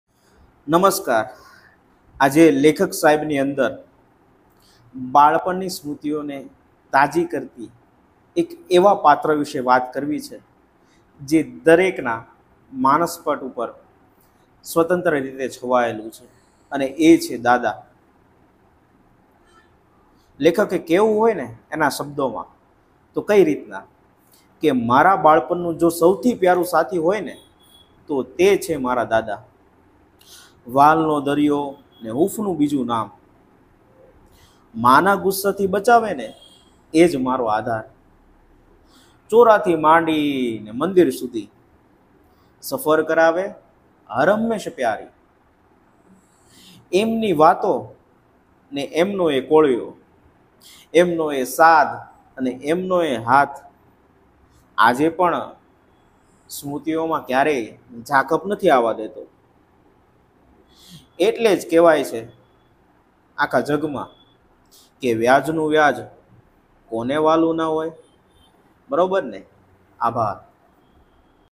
બાળપણ ની યાદો - "દાદા" | lekhak ni vat ne dada ni yad
દાદા એટલે બાળપણની માલિપા સંતાયેલી એક એવી ઓળખાણ કે એને ક્યારેય નામ કે ઠેકાણું ના આપવું પડે એનો તો મનથી જ આનંદ લેવાય હો વાલા.
પોસ્ટ : @_lekhak_saheb & @kthummar
રજુઆત & લેખન : @ghanshyam.c.thummar